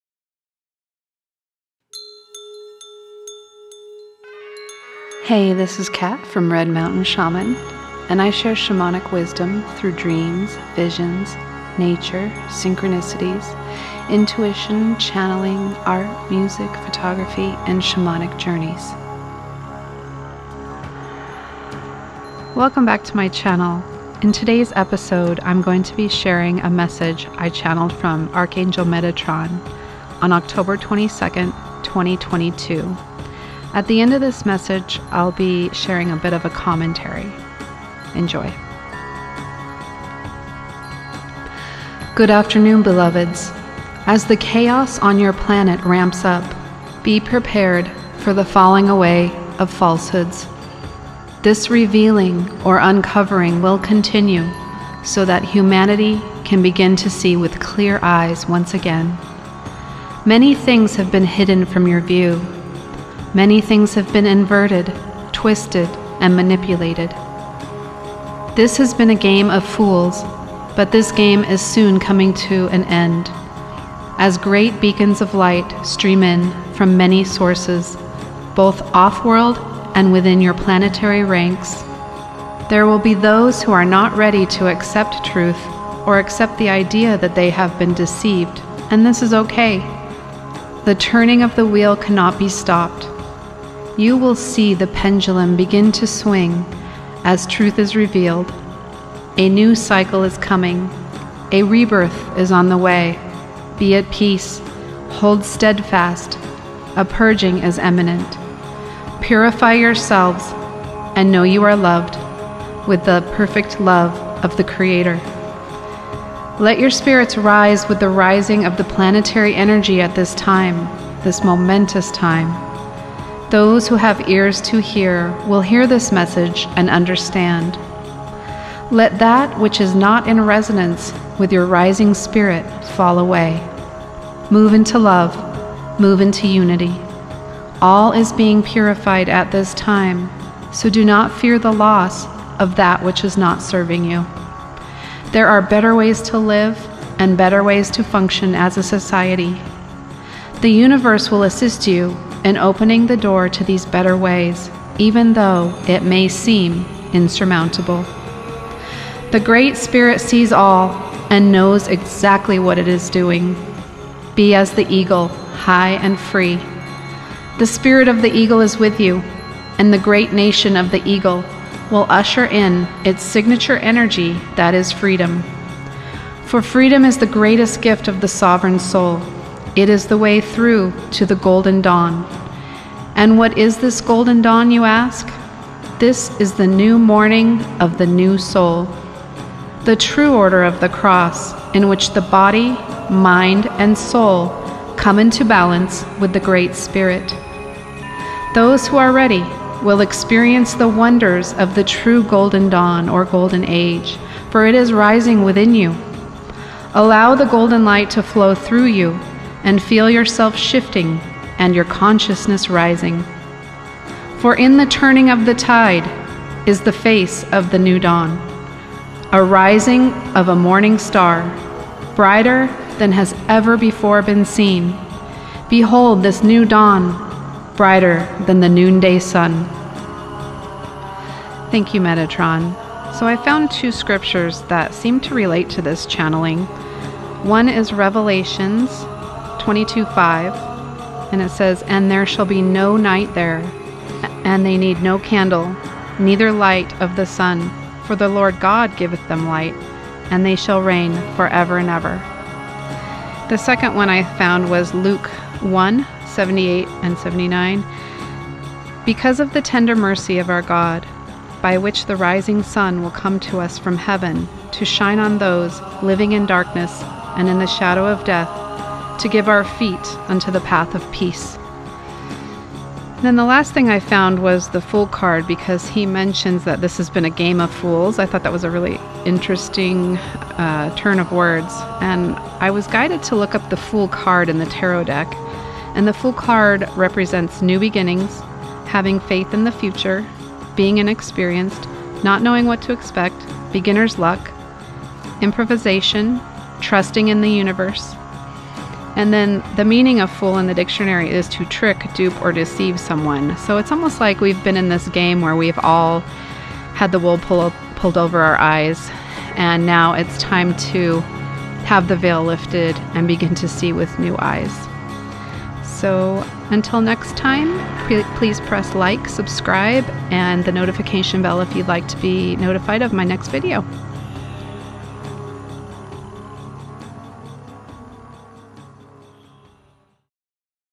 Channeling Metatron Oct 22/22 portal energy update
Channeled message from Archangel Metatron with short commentary afterwords.